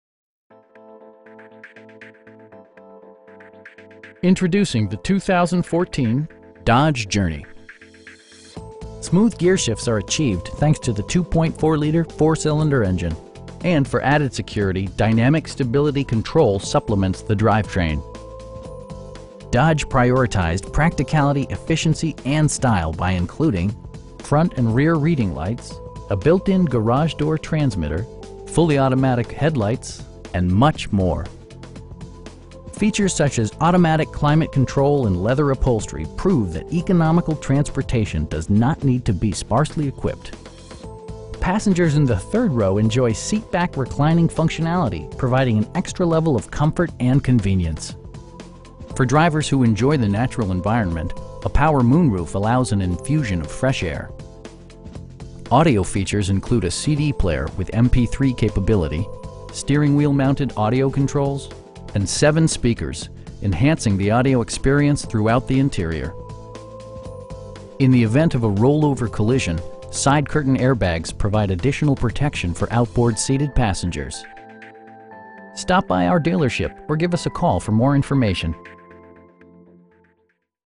2014 Dodge Journey SXT in San Diego, CA 92111
Rancho Chrysler Dodge Jeep RAM
8010 Balboa Avenue

Introducing the 2014 Dodge Journey. Smooth gearshifts are achieved thanks to the 2.4 liter 4 cylinder engine, and for added security, dynamic Stability Control supplements the drivetrain. Dodge prioritized practicality, efficiency, and style by including: front and rear reading lights, a tachometer, a built-in garage door transmitter, heated seats, fully automatic headlights, power door mirrors, and much more. Features such as automatic climate control and leather upholstery prove that economical transportation does not need to be sparsely equipped. Passengers in the third row enjoy seat back reclining functionality, providing an extra level of comfort and convenience. For drivers who enjoy the natural environment, a power moon roof allows an infusion of fresh air. Audio features include a CD player with MP3 capability, steering wheel mounted audio controls, and 7 speakers, providing excellent sound throughout the cabin. Rear LCD monitors provide entertainment that your passengers will appreciate no matter how far the drive. In the event of a rollover collision, side curtain airbags provide additional protection for outboard seated passengers. Our aim is to provide our customers with the best prices and service at all times. Stop by our dealership or give us a call for more information.